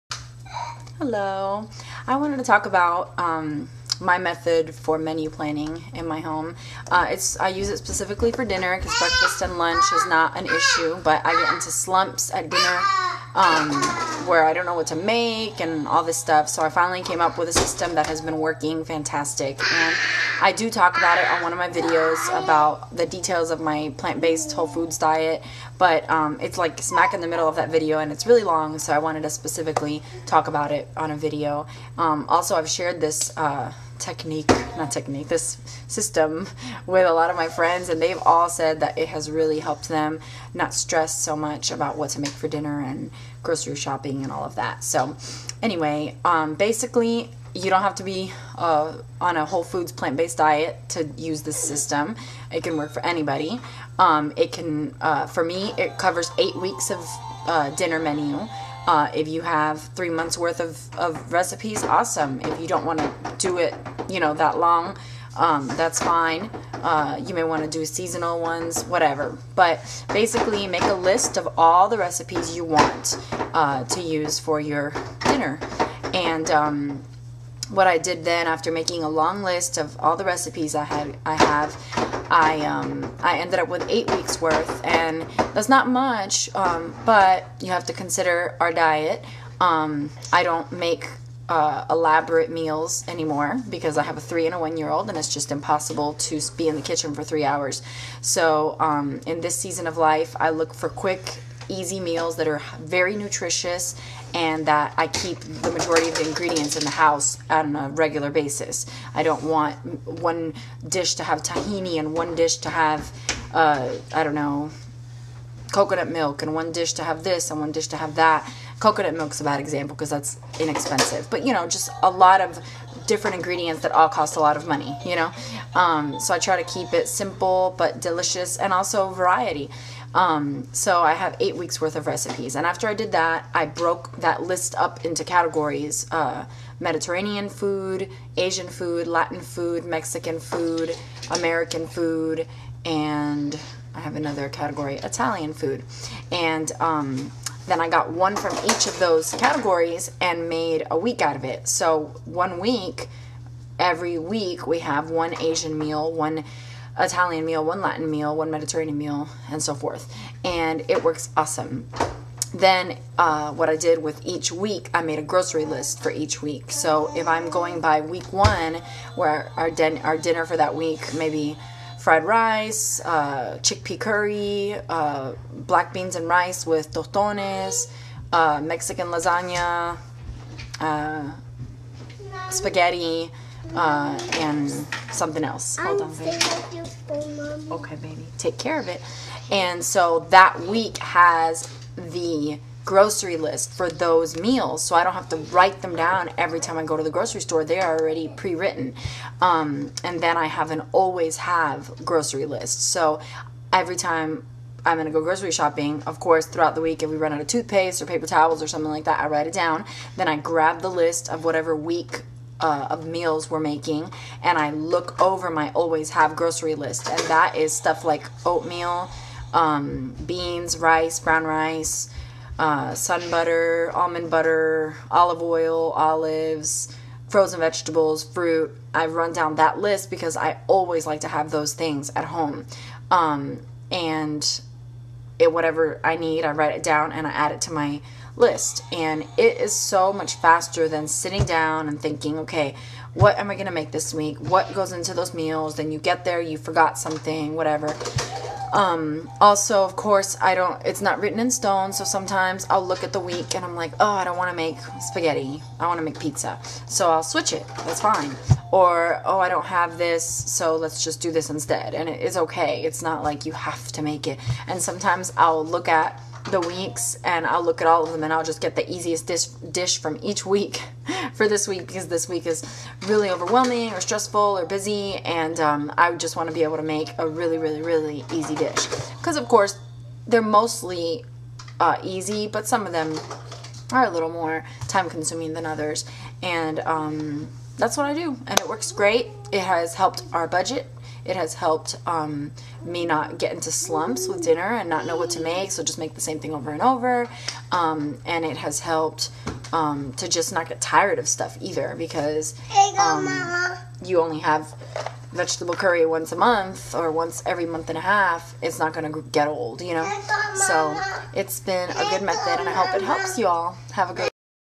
Meal Planning System
I describe how I plan my dinner menu.